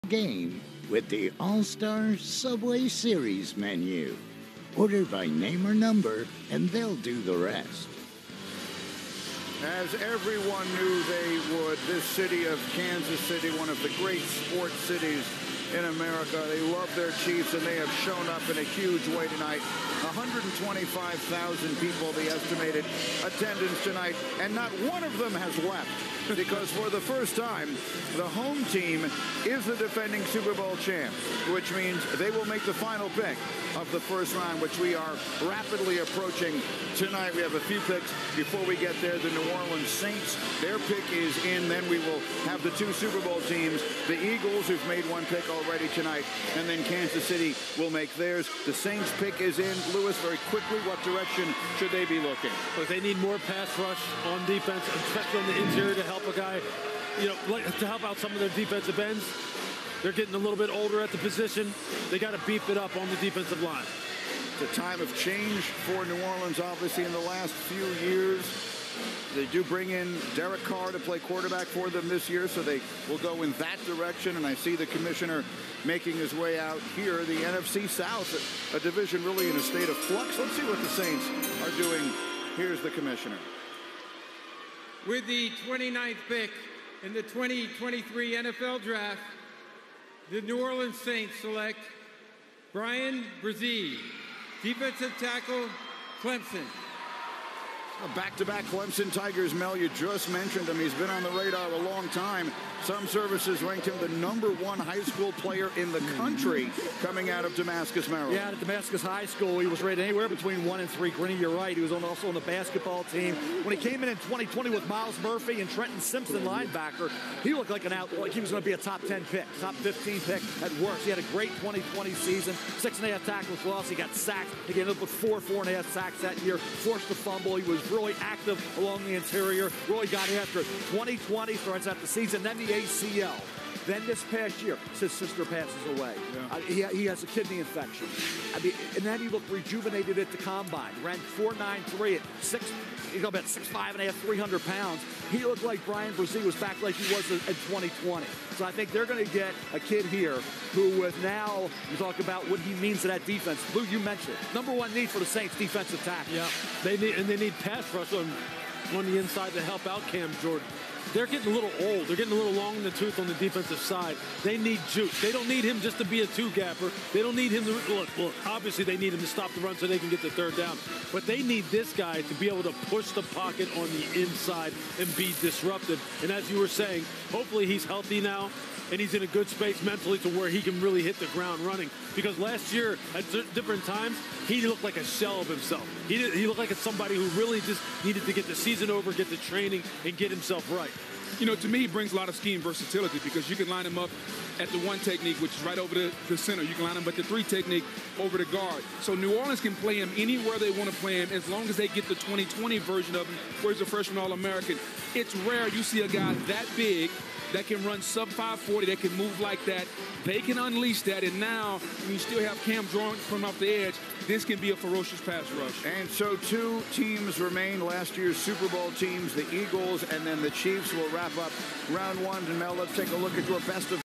New Orleans Saints have drafted Bryan Bresee!
Good boost to their pass rush.